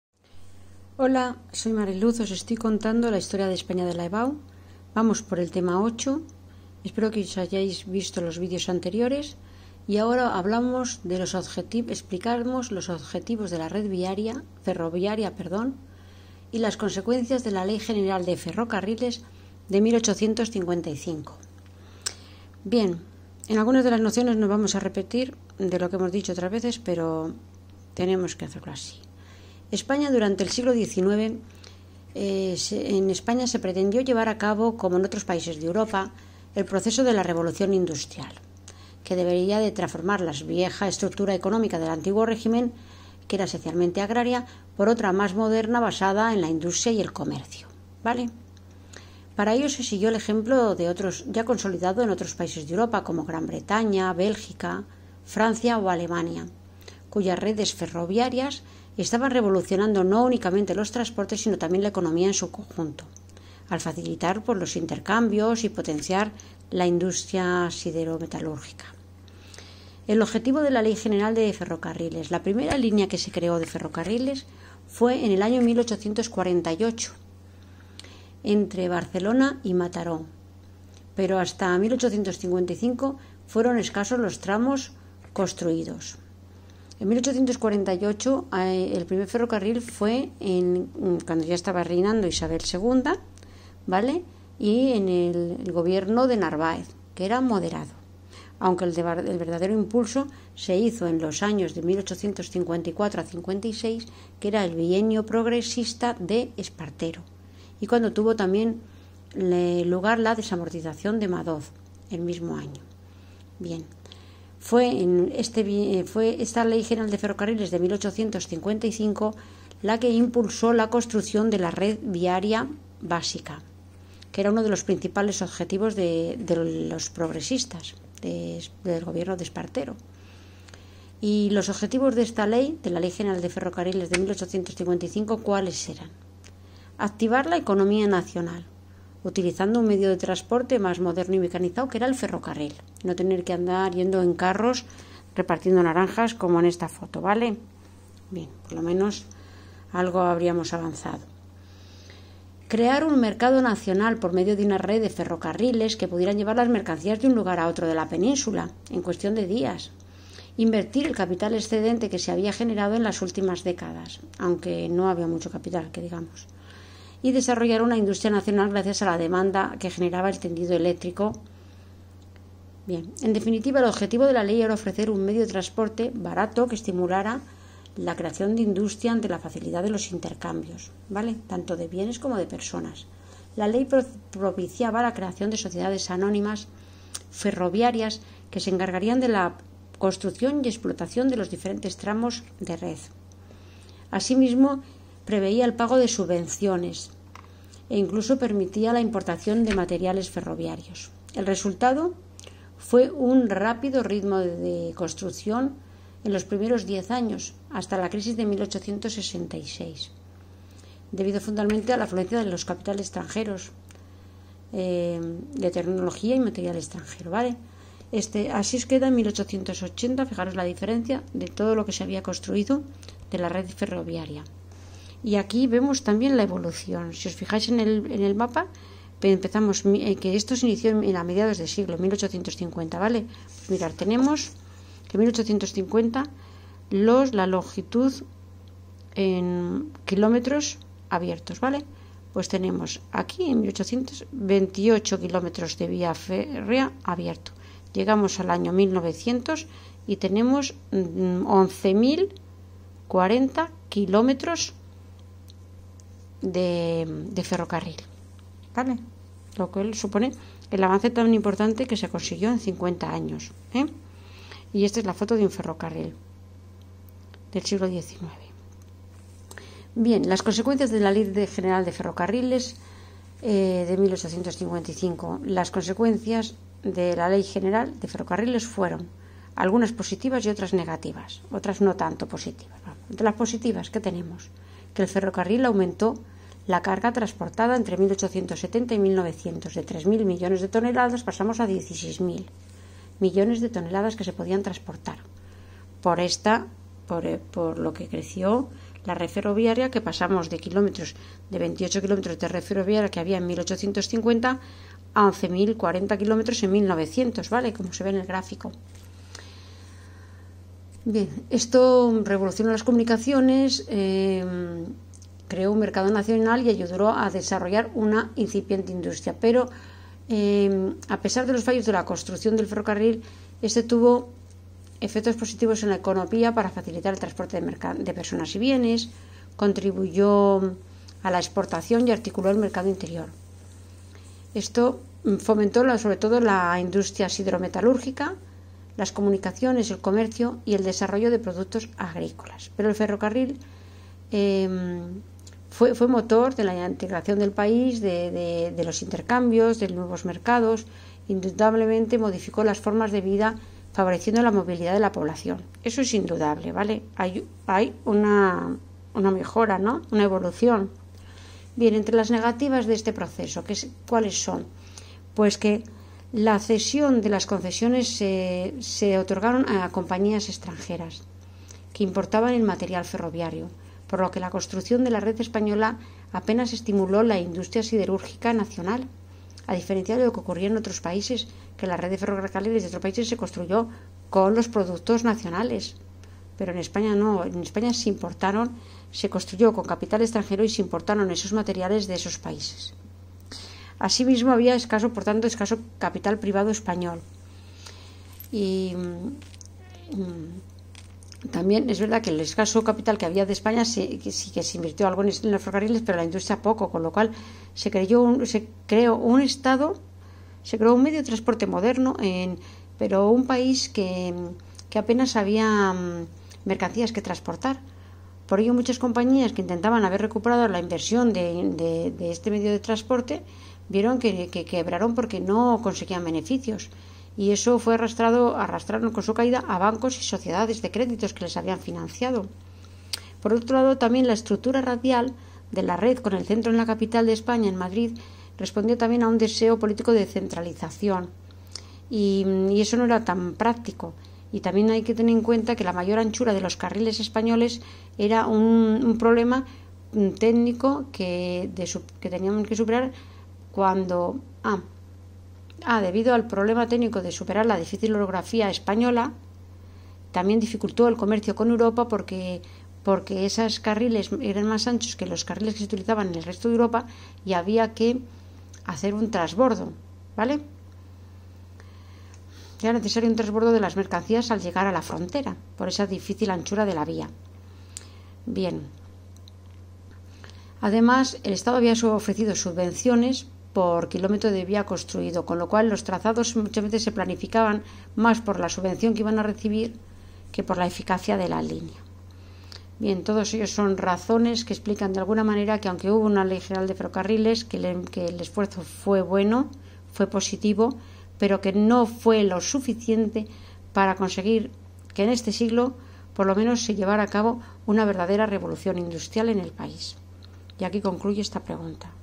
HISTORIA DE ESPAÑA DE LA EVAU/EBAU: BLOQUE 8, TEMA 4, la Ley General de Ferrocarriles de 1855
Explica los objetivos de la red ferroviaria y las consecuencias de la Ley General de Ferrocarriles de 1855